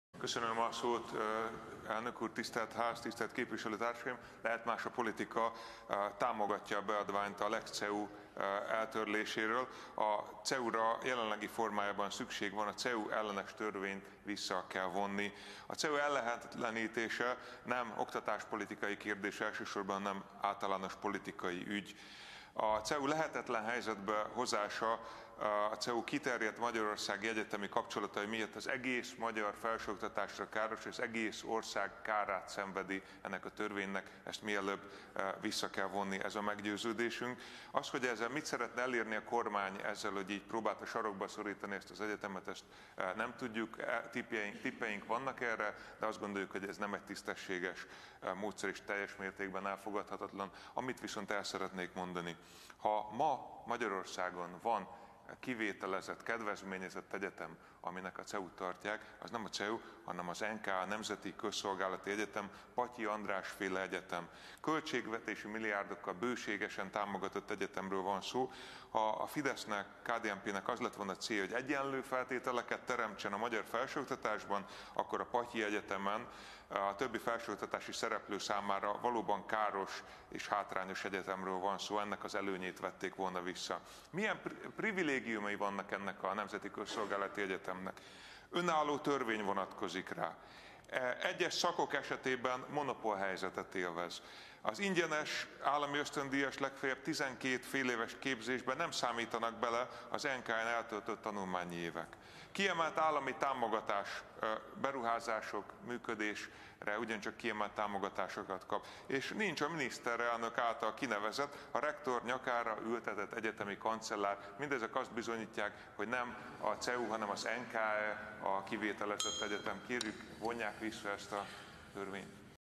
Nem a CEU, hanem az NKE a kivételezett egyetem (LMP - Politika)
Az egész ország kárát szenvedi a Lex CEU-nak. Ha van ma Magyarországon olyan egyetem, amely nem egyenlő feltételekkel indul, akkor az nem a CEU, hanem a Nemzeti Közszolgálati Egyetem.